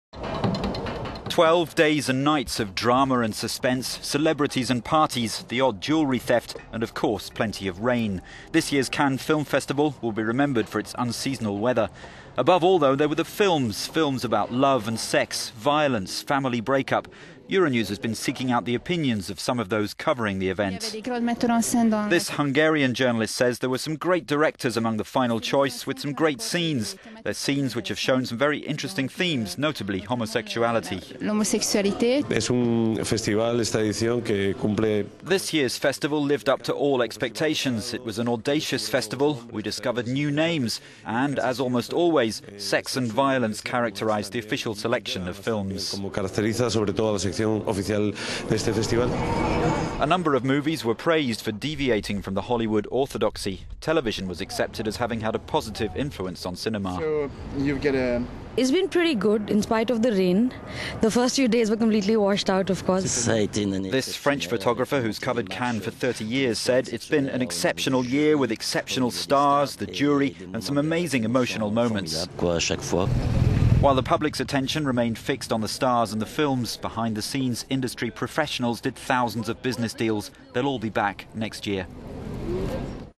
Cannes 2013: suspense, stars, thefts... and movies
Twelve days and nights are drawing to a close: days and nights of drama and suspense, celebrities and parties, the odd jewellery theft - and of course, plenty of rain.

The 66th Cannes Film Festival will be remembered for its unseasonal weather.

Above all there were the films: films about love, sex, violence, family breakup, and other emotional anguish.

Euronews has been seeking out the opinions of some of those covering the event.

"There were some great directors among the final choice, and so there were some great scenes. They're scenes which have shown some very interesting themes, notably homosexuality," said Katalin Venczel of the Hungarian news agency MTI.

"This year's festival edition lived up to all expectations, it was an audacious festival, we discovered new names, and as almost always, sex and violence characterised the official selection of films," was the assessment of David del Rio from Mexico's Agencia Notimex.

A number of movies were praised for deviating from the Hollywood orthodoxy. Television was accepted as having had a positive influence on cinema.

Alan Schoenauer, a freelance French photographer covering Cannes since 1982, said:
"It's been an exceptional year with exceptional arrivals up the steps each night, and with the jury, the stars, and some amazing moments of emotion each time."

While the attention of the public at large remained fixed on the stars and the films on offer, behind the scenes industry professionals did thousands of business deals.

They will all be back next year.